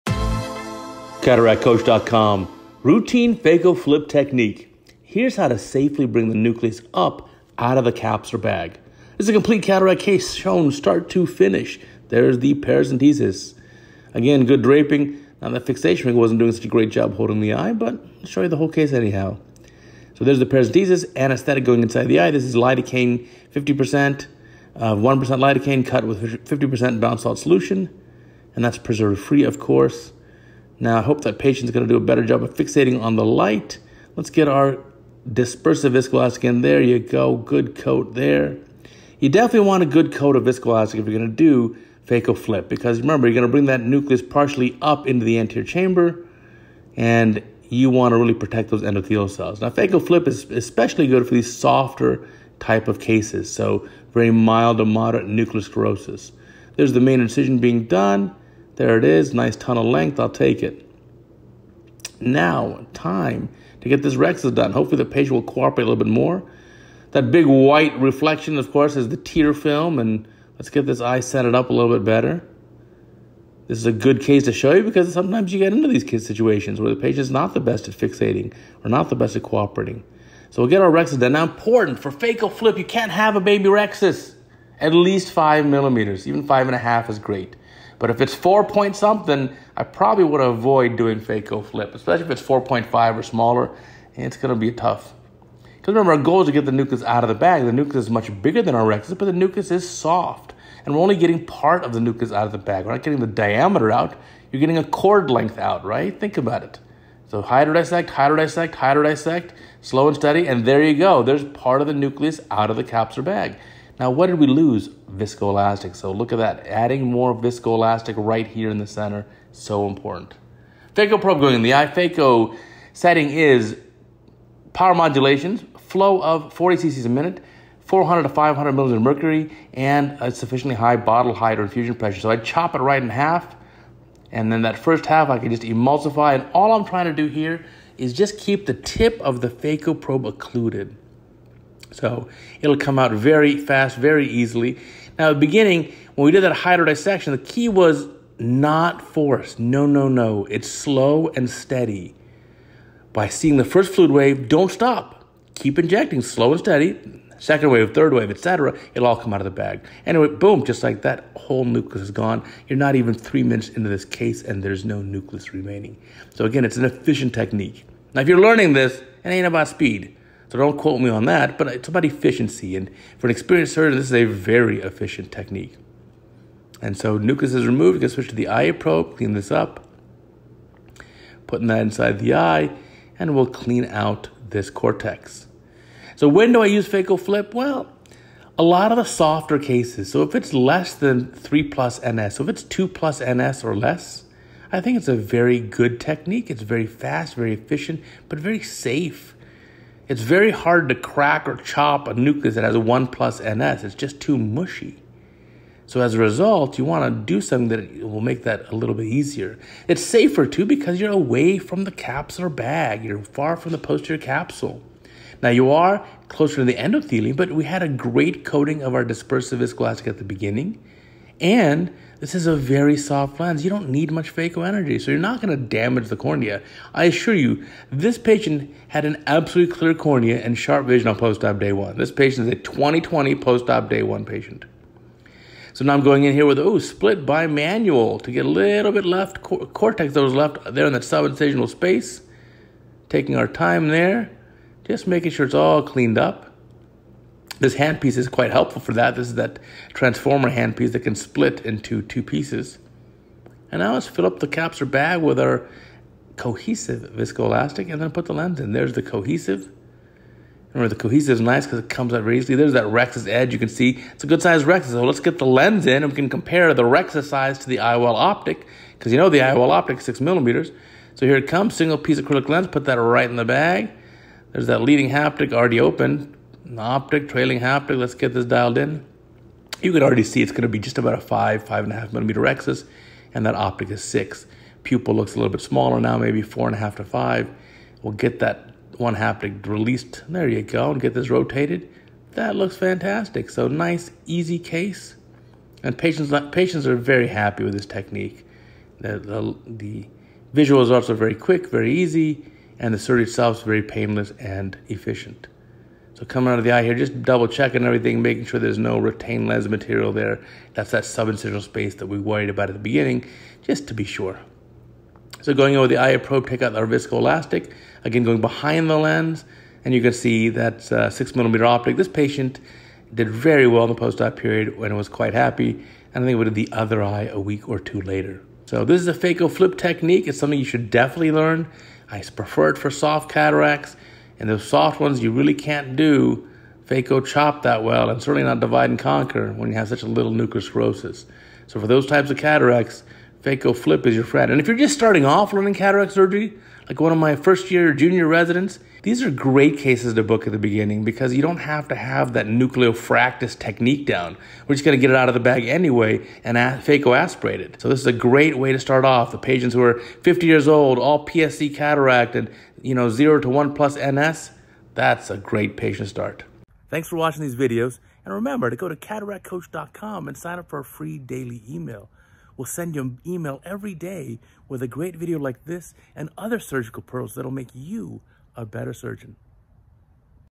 CataractCoach 1166: routine phaco flip technique
In our Complete Cataract Case series, we present the technique of phaco flip. This is a great technique for a softer nucleus with less nuclear sclerosis where it is difficult to chop a mushy nucleus. This involves creating a sufficiently large capsulorhexis (at least 5 mm in diameter) and then using gentle but persistent hydro-dissection to partially prolapse the nucleus out of the capsular bag. The technique is explained in detail in this video and to learn the low force level needed for hydro-dissection, please refer to this prior video.